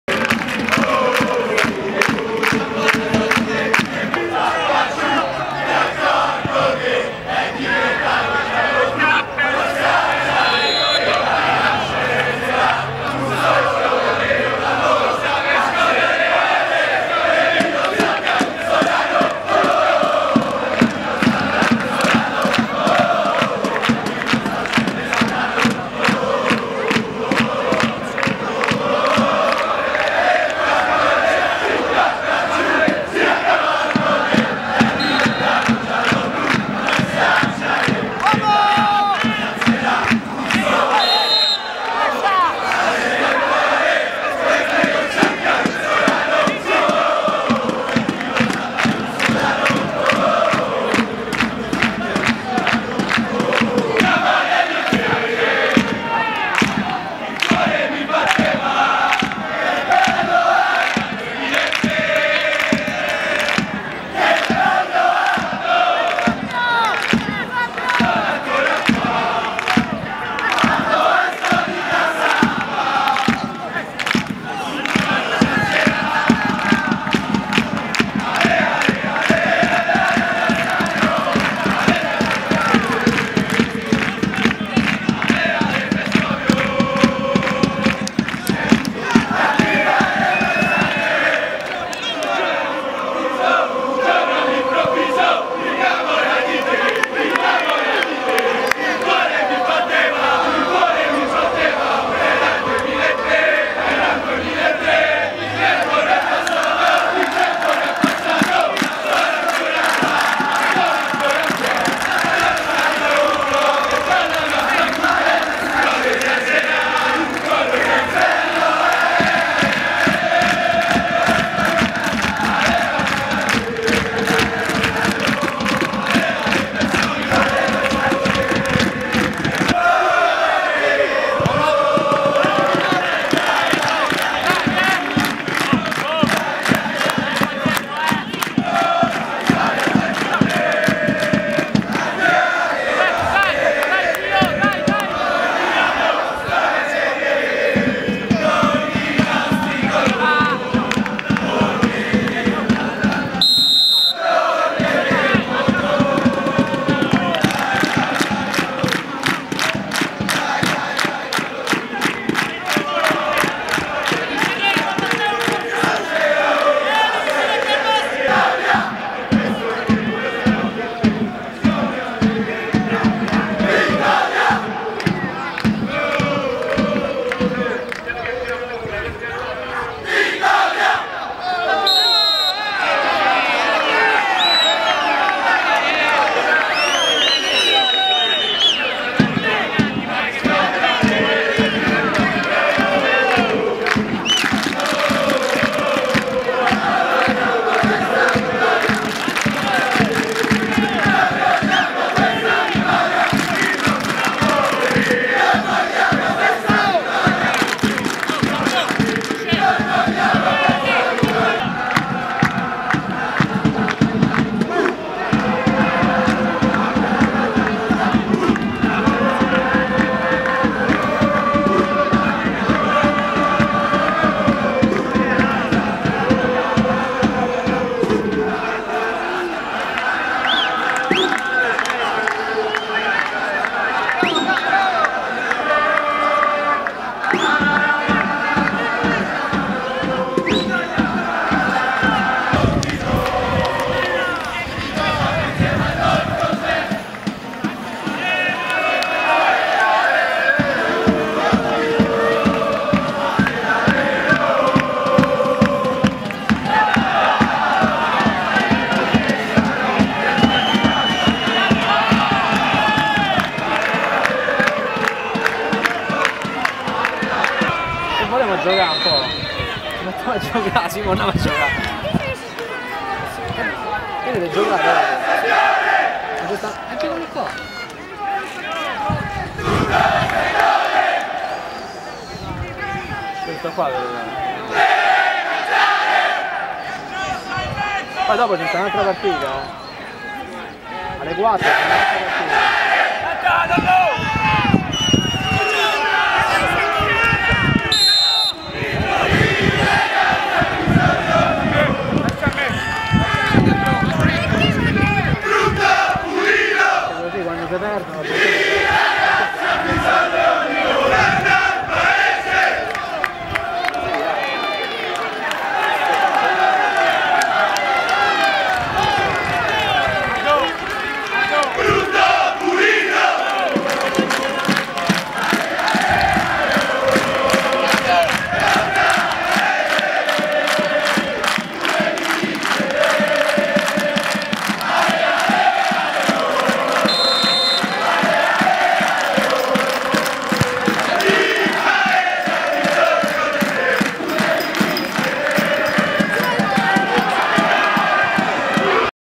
Ultras Arce in azione contro l'Atletico Vescovio, Playoff Promozione Laziale 2014/15
Qualsiasi partita, in qualsiasi categoria, laddove sia in palio qualcosa di importante, riesce sempre ad offrire una gamma di emozioni in più, non fosse altro per questioni di puro agonismo. Quando poi sugli spalti ci sono tifosi partecipi all'evento in maniera attiva, l'ambiente diventa spesso un evento nell'evento. Tifo, colore e calore di Atletico Vescovio-Arce in questi video del nostro inviato Simone Meloni.